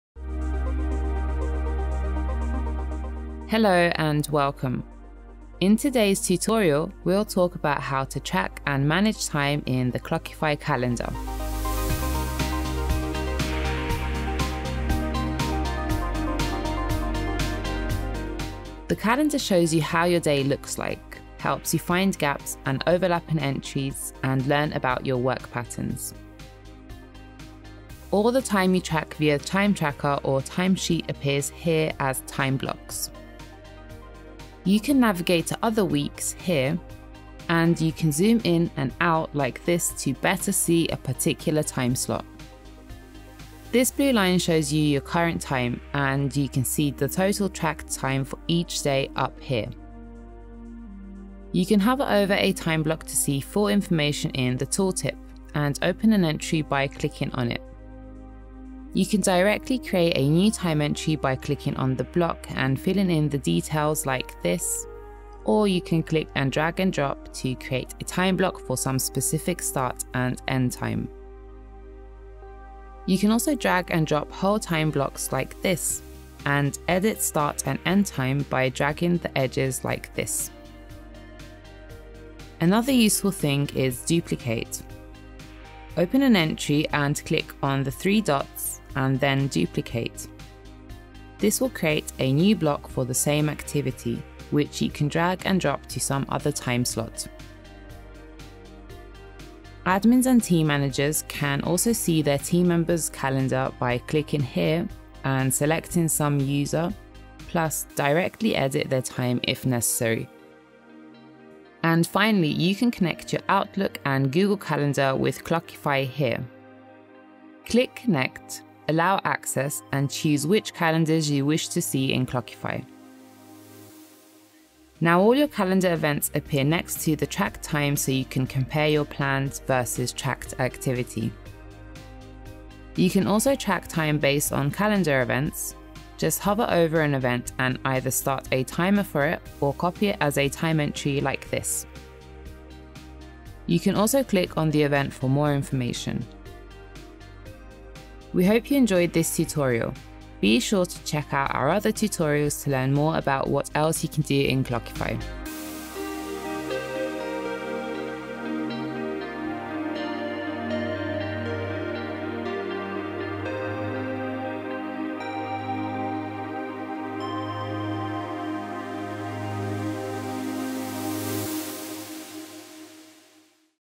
Clockify Tutorial: Calendar
How to use Calendar in Clockify, track and organize your time and integrate Google Calendar or Microsoft Outlook.

0:00 Intro
0:19 Viewing
0:51 Editing
1:51 Integrations
2:27 Outro

Clockify is free time tracking software that lets you and your team track time with one click.